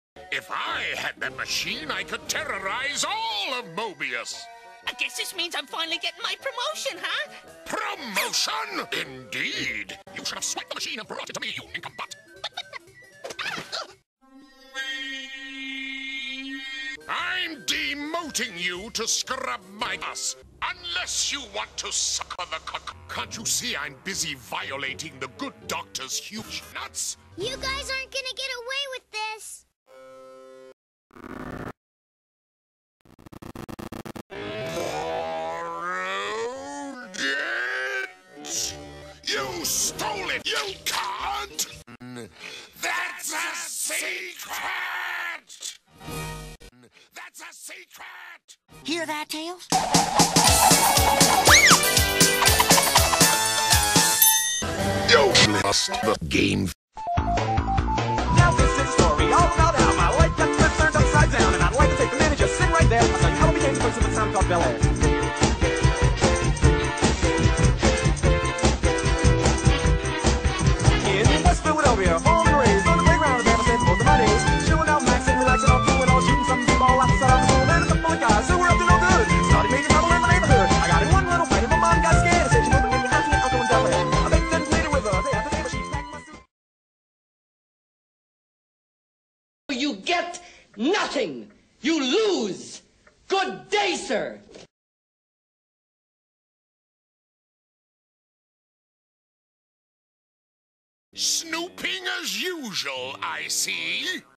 Robotnik's Jesus Machine
I guess you could look at this as my 20th subscriber special.  Not as good as I thought it would be, but still my best yet.  Windows Movie Maker likes to cut words and sounds out.  Making them quieter seems to work a little bit.  Sorry, the tennis might take a little longer because of this.

The song at the end wasn't made by me, it was put together by RecFreeMedia with obvious sources.